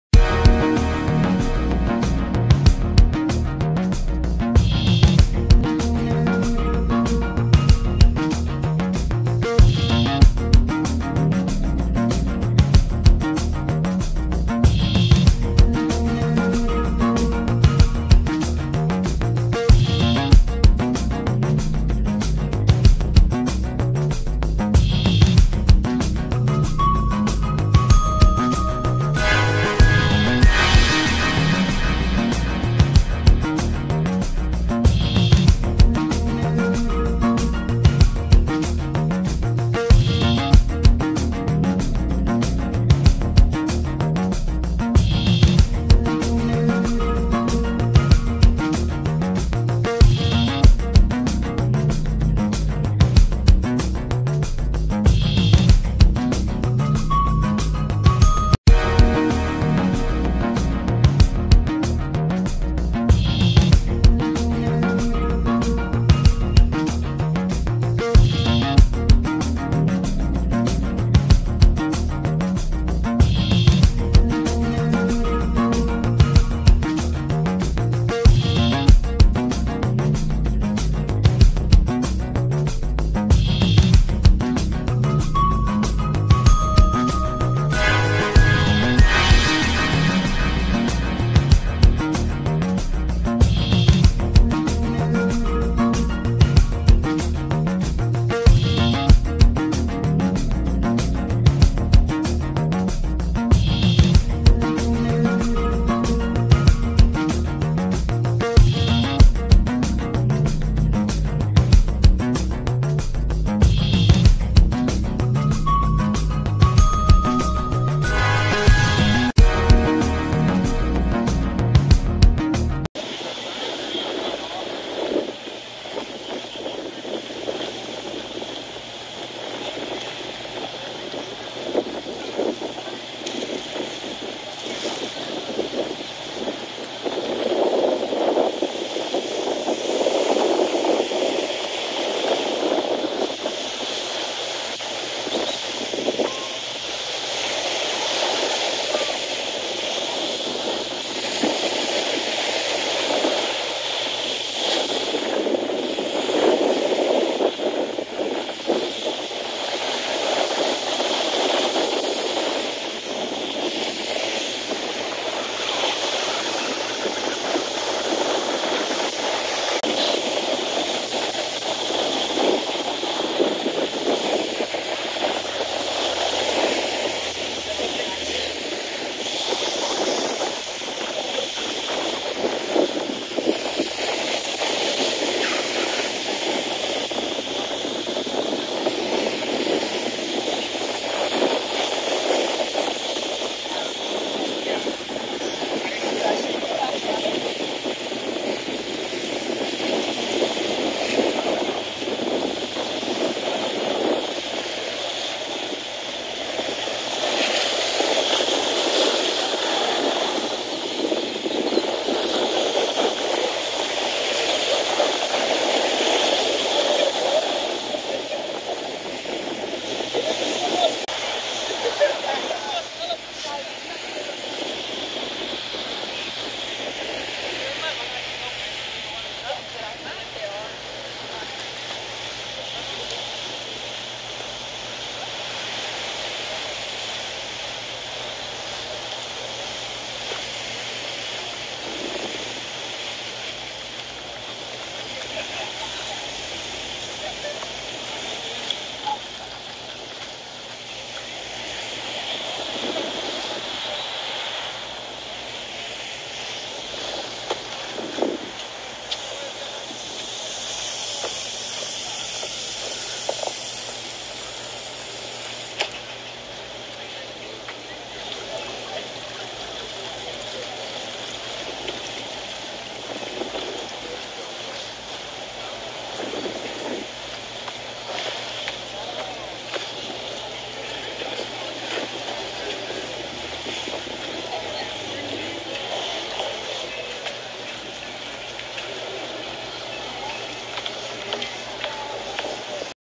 Hilton PDC Luxury All Inclusive Resort Adults Only
Hilton PDC luxury all inclusive resorts adults only with 513 rooms 8 restaurants 5 bars and 3 outdoor pool